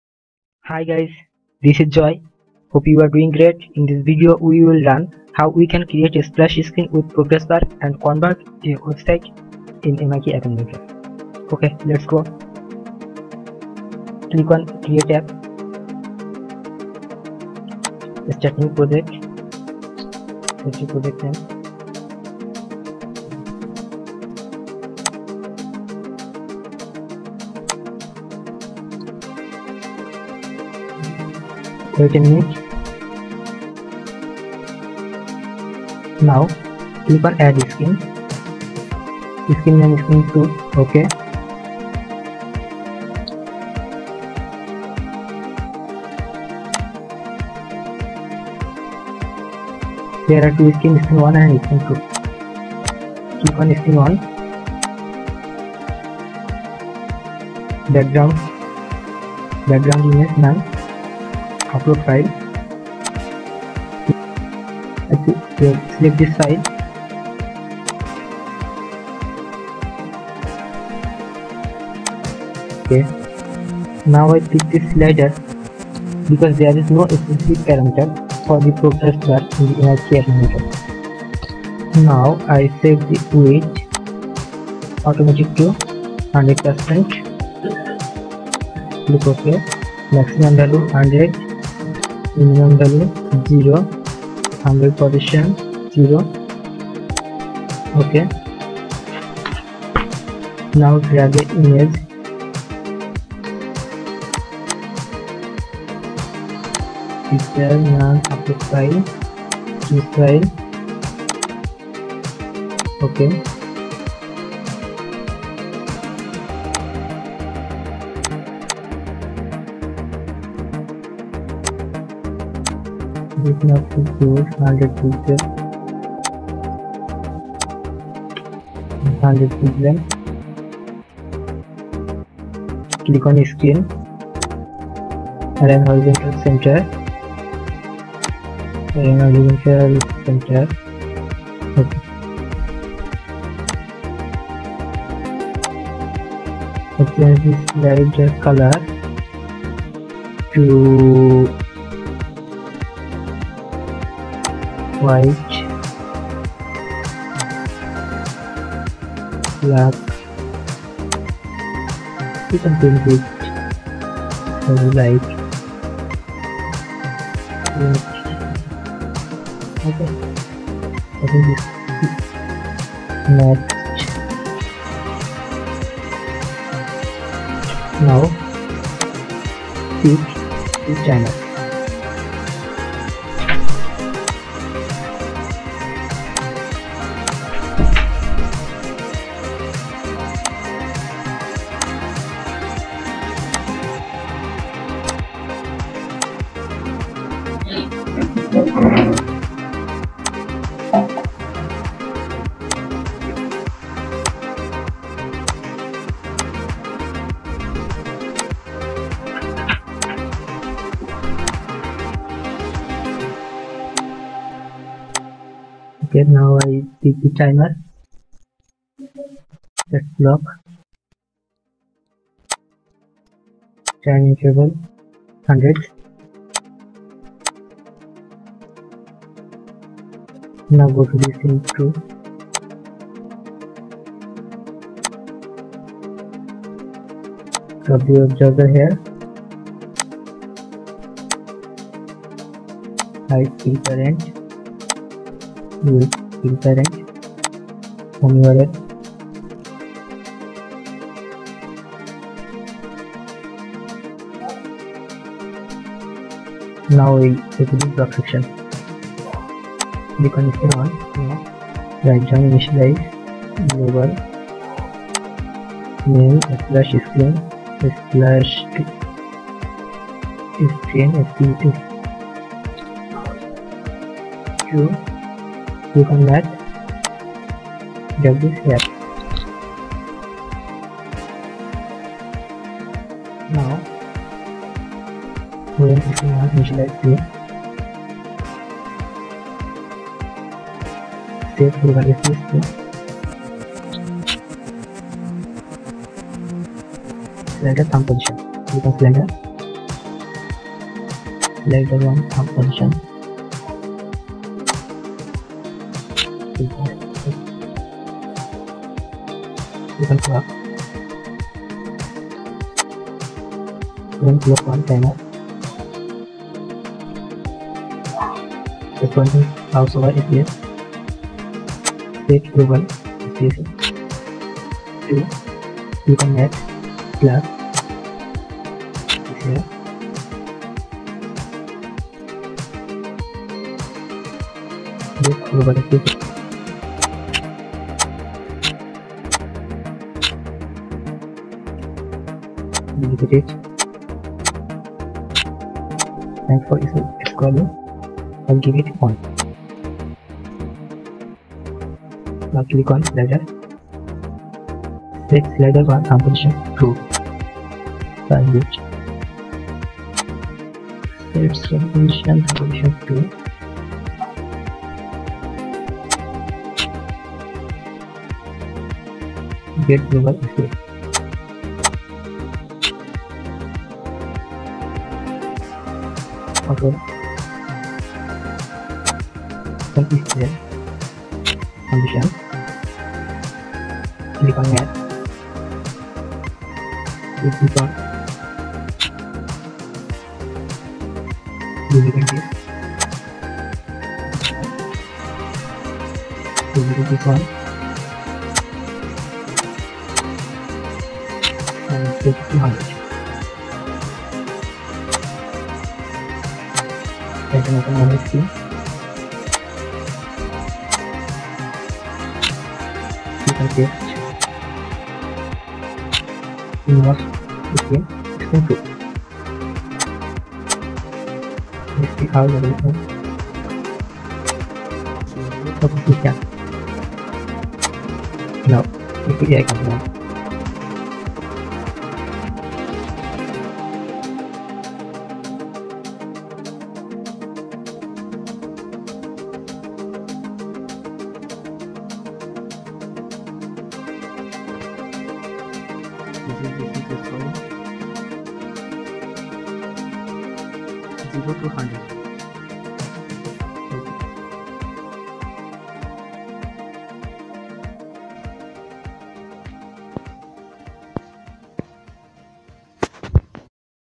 MIT App Inventor Tutorial - 6 : How to convert website into android app with splash screen
How to convert website into android appIn This video we will learn how we can create splash screen with progress bar and convert a website into android application in mit app inventor.
At first we create a new project. Then create a extra another screen name "screen 2".
Now select the screen 1.
Step-1: click on Background image and upload a image.
Step-2: Drag Slider.
Step-3: Set width automatic to 100 percent.
Step-4: Max value to 100 and Min Value to 0 .
Step-5: Thumb Position to 0.
Step-6: Now drag a image and upload here your website logo.Now resize it showing in video.
Step-7: Drag a clock and set time interval to 100

Now select the screen 2.
Step-1: Drag browser.
Step-2: Set your destination site url.
Step-3: Invisible the screen name.
now go to the block. And rearrange the block as shown in video.